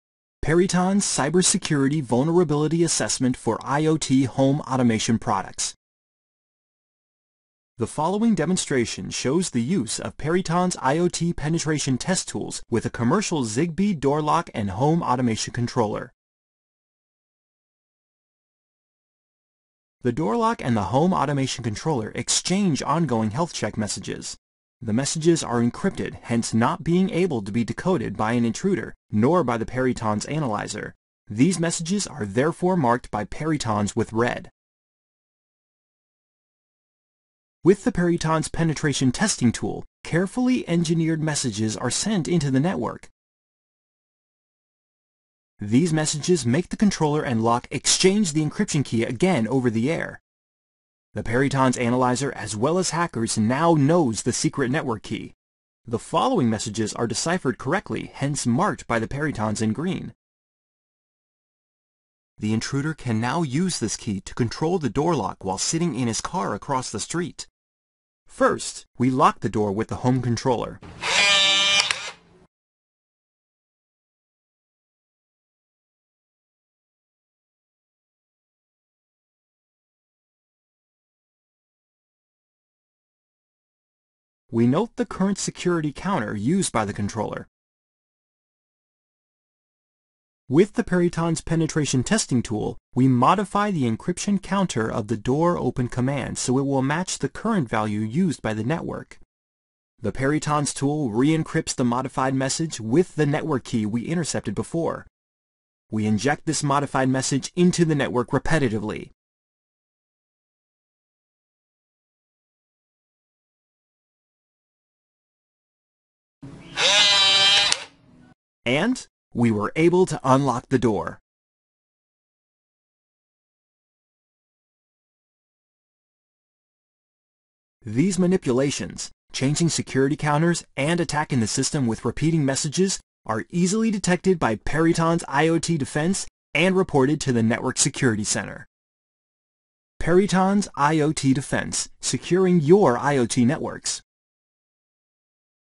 IoT Penetration Testing Example
IoT cyber security vulnerability assessment for ZigBee Home Automation system with Perytons Penetration Tools